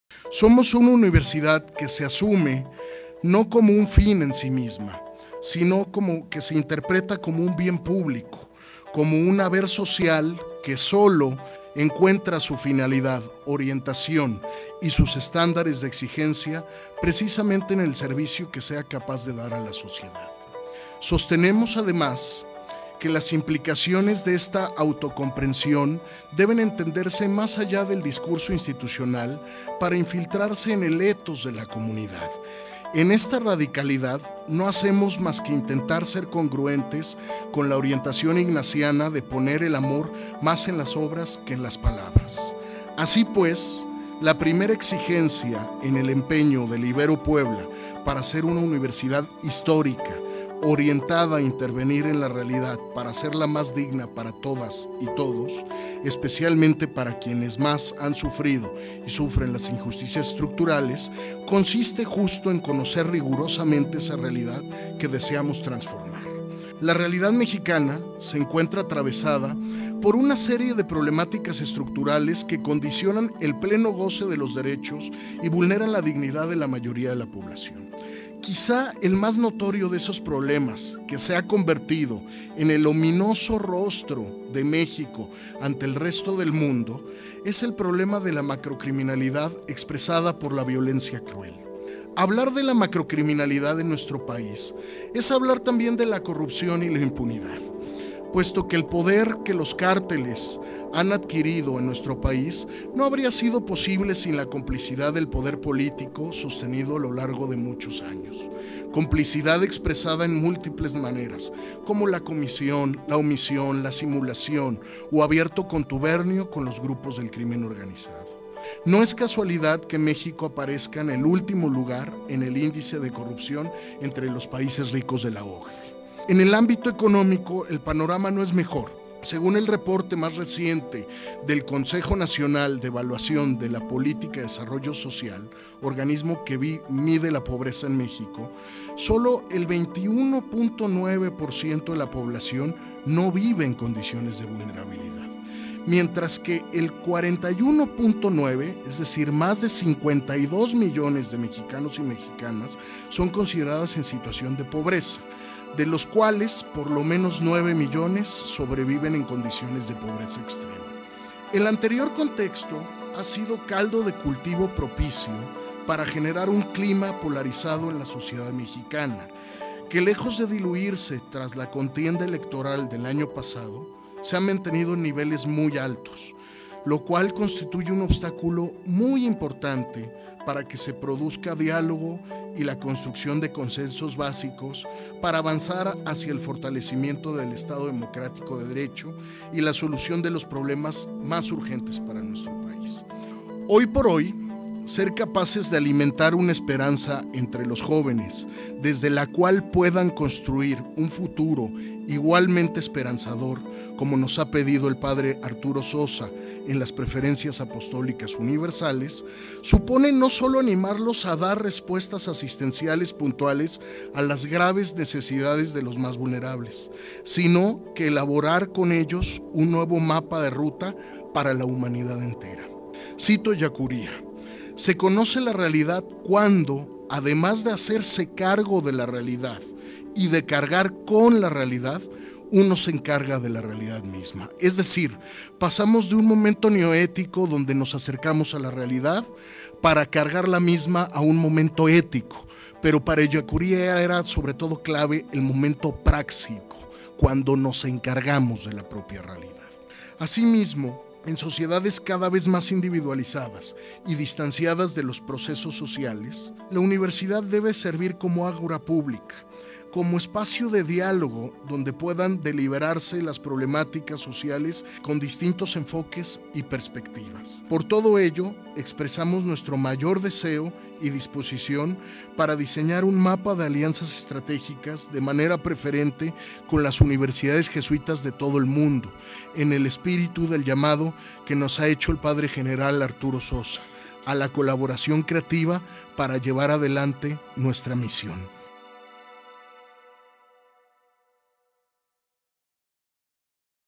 International Education Conference of Jesuit Higher Education Institutions
"La universidad debe servir como ágora pública, como espacio de diálogo donde puedan deliberarse las problemáticas sociales con distintos enfoques". Fuimos sede del International Education Conference of Jesuit Higher Education Institutions.